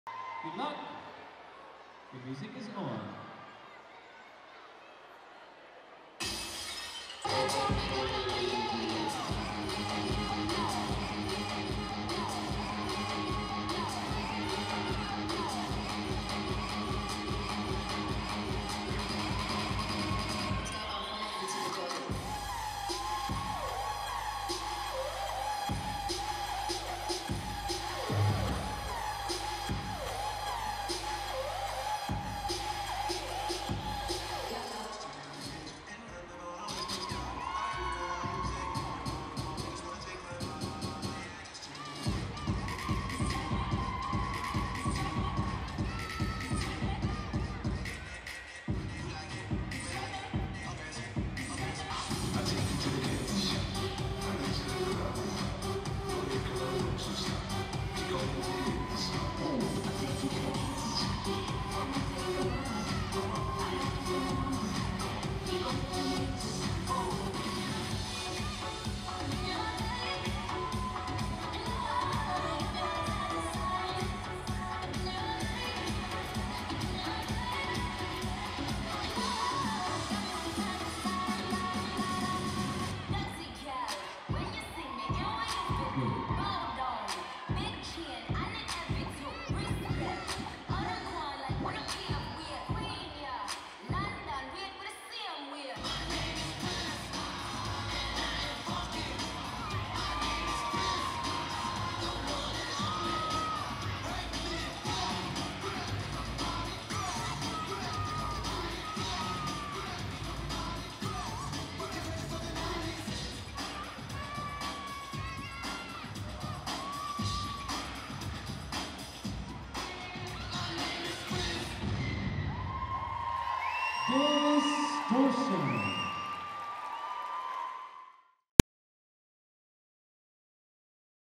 DISTORTION - BDO East Anglia Street Dance Championship 2017
Team performance from the BDO East Anglian Street Dance Championships 2017 at the Bushfield Sports Centre in Peterborough.
Filmed and edited by JHH Media.